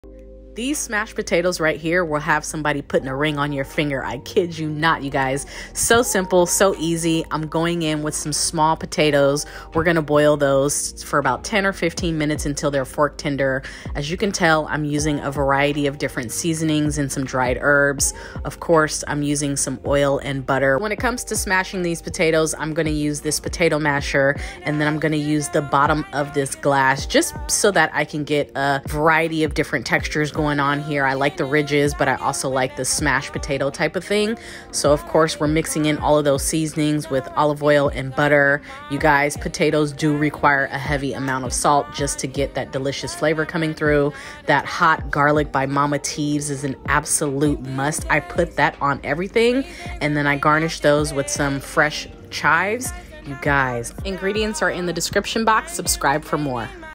CRISPY 😱🥔 SMASHED POTATOES | BUTTER GARLIC CHIVES 🧄🧈🌱
Ingredients:
1-2 lbs (small bag) red or multi-colored potatoes
1 tbsp butter (melted)
1 tsp extra virgin olive oil
1 tbsp fresh chives

Seasonings of your choice: (1/4 tsp each or less)
Kinders’ Salt and Pepper
Kinders’ Mediterranean seasoning
Garlic salt
Onion powder
Paprika
Italian dried herbs
Dried parsley
1-2 tbsp Mama Teav’s Mild Garlic

Instructions:
Rinse potatoes and boil in heavily salted water for about 12-14 minutes until fork-tender.
Strain and let cool for a few minutes.
Mix melted butter, olive oil and all your favorite seasonings together well.
Lightly oil a cookie sheet, assemble the strained and cooled potatoes on the sheet -
Using a potato masher and/or a small drinking glass, carefully smash the potatoes to your desired texture.
Use a brush to coat the potatoes well. Add any extra seasoning to the potatoes and the pop them in the oven for 20 mins.
Broil on HIGH 3-5 mins watching closely to avoid burning potatoes.
Garnish with chives and any other toppings that tickle your fancy 😘